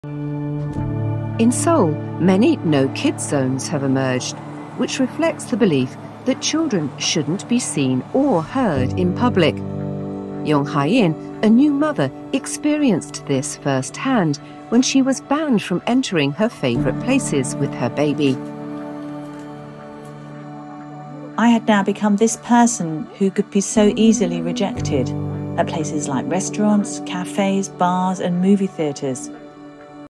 no kids zones
In Seoul, 'No Kids Zones' are becoming common. The idea that children should stay hidden and quiet is creating this problem. Yong Hye-in couldn't visit her favourite places with her new born baby, which made her sad. This is worrying because it’s unfair to families.

In this lesson, you will practise:
+ language for describing changes
+ vocabulary about politics, society and family life
+ listening to a Standard Southern British English accent